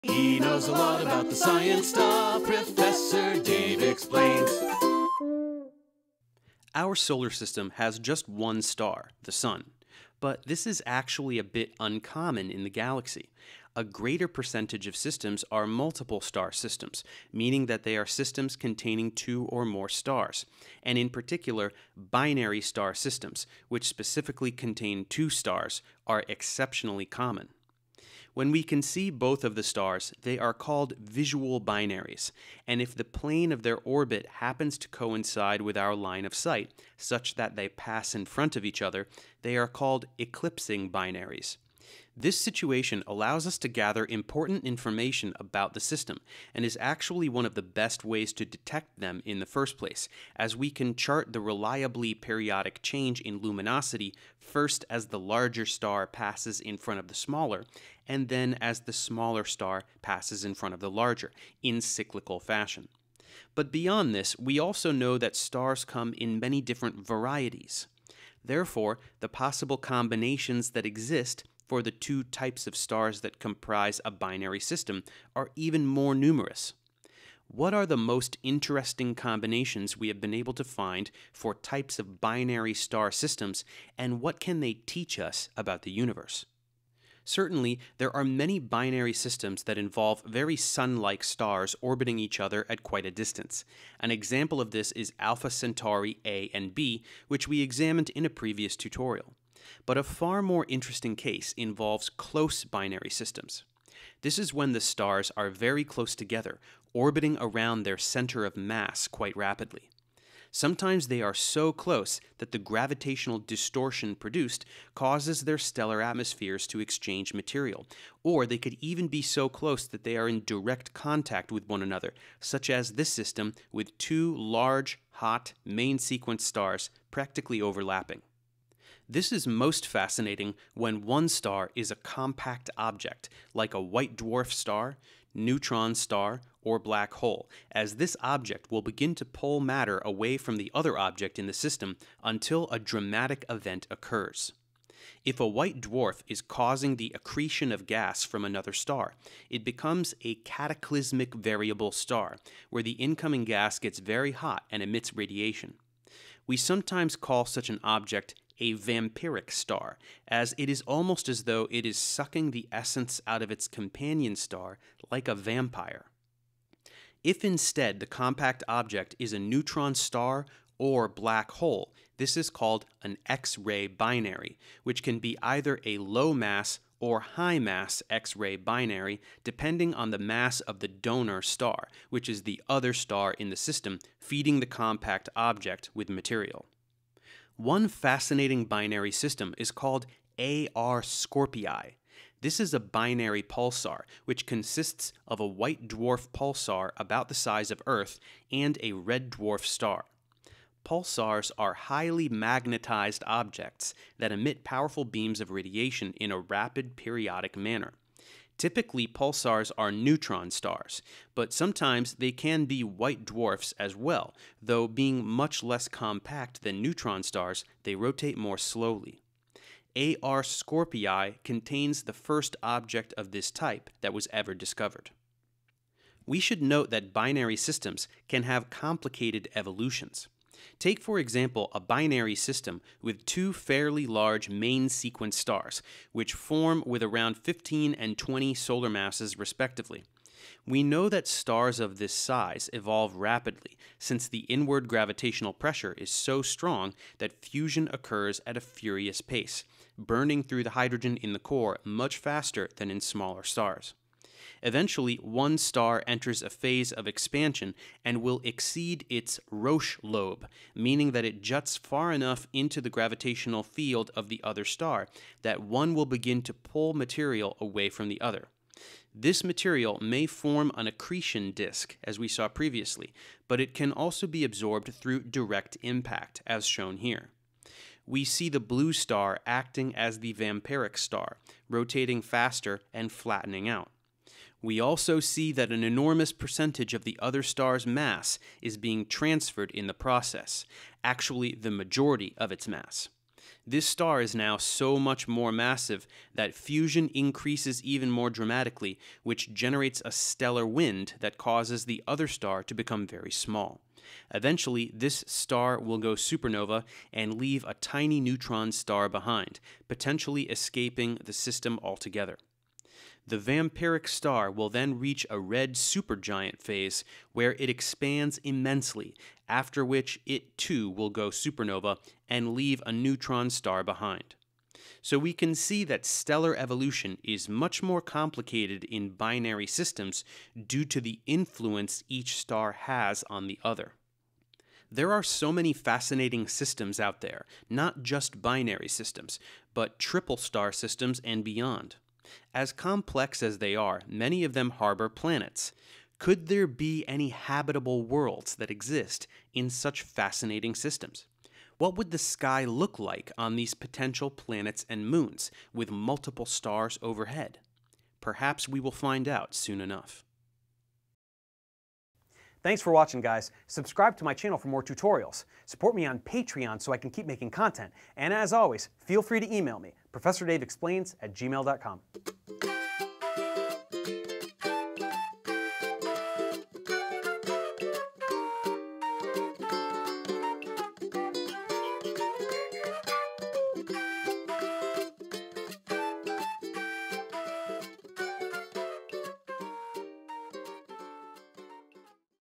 Types of Binary Star Systems
Our solar system has just one star in it, the sun. But this is actually not the most common situation for systems. Most systems are multi-star systems, with binary systems being extremely common. These are systems where two stars orbit around their center of mass, and some of them even have planets. But we also know that stars come in so many varieties, due to their differing masses, and they leave behind different remnants. For this reason, there are a great many possible combinations of star types that produce different kinds of binary systems. What happens when you have a black hole and a main sequence star as a binary system? Or a pulsar and a red dwarf? How do the stars influence each others evolution? It's intense! Let's take a look.